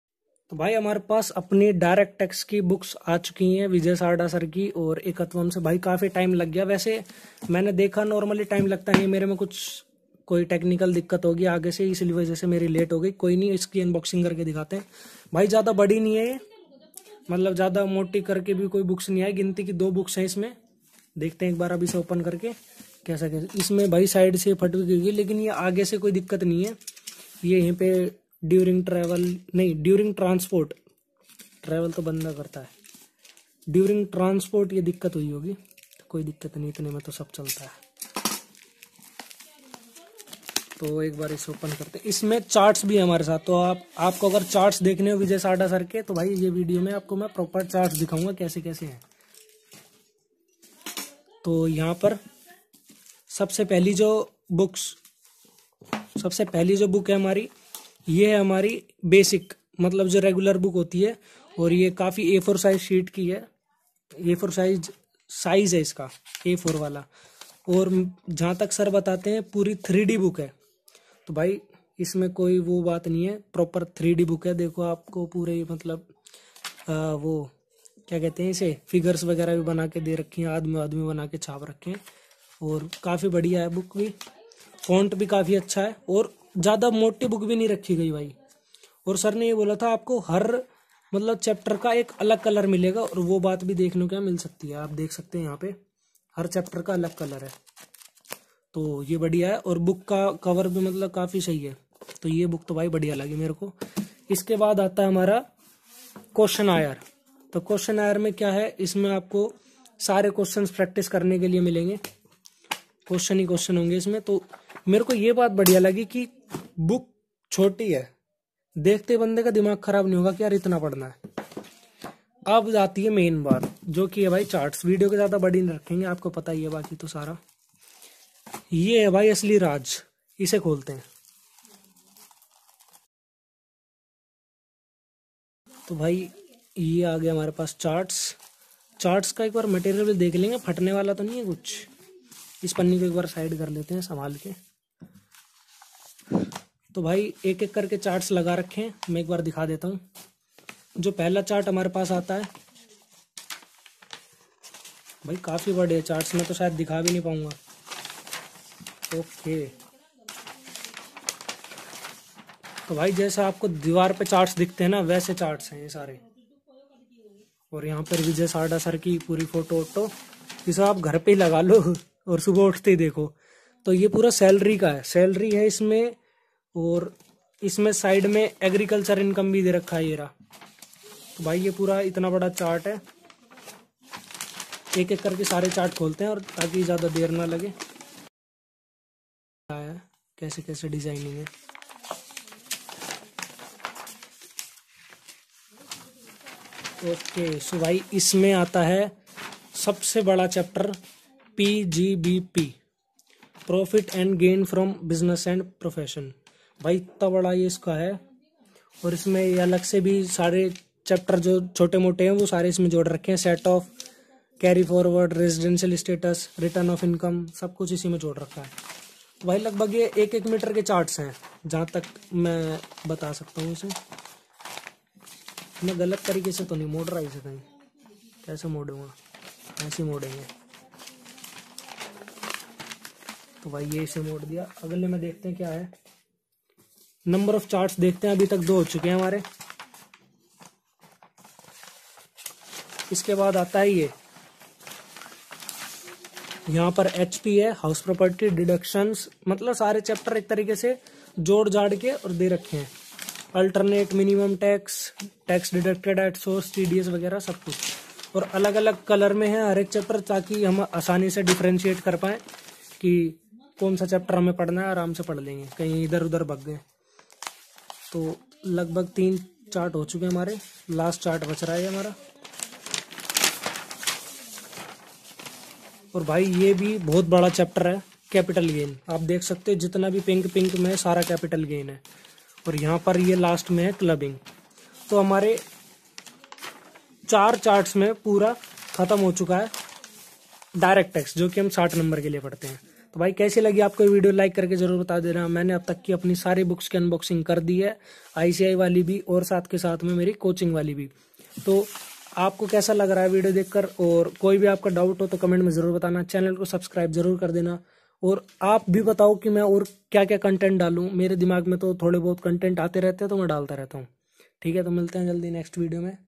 CA Vijay sarda sir DT Books Unboxing| Income Tax books | Charts | Complete review
The video titled "CA Vijay Sarda Sir DT Books Unboxing| Income Tax Books | Charts | Complete Review| @CAVijaySarda" is a comprehensive review and unboxing of income tax books and charts by CA Vijay Sarda. In the video, CA Vijay Sarda, a well-known chartered accountant, unboxes a set of income tax books and charts and provides a detailed review of each item.

Throughout the video, CA Vijay Sarda explains the contents of each book, its relevance, and its practical application in the field of income tax. He also highlights the unique features of each chart and its significance in making complex tax concepts easier to understand.

In addition to providing a detailed review of each item, CA Vijay Sarda also shares his personal experience with these books and charts, highlighting their effectiveness in his own practice.

Overall, this video is an informative and insightful review of income tax books and charts by an expert in the field. It is an excellent resource for anyone interested in improving their understanding of income tax concepts and staying up-to-date with the latest developments in this area.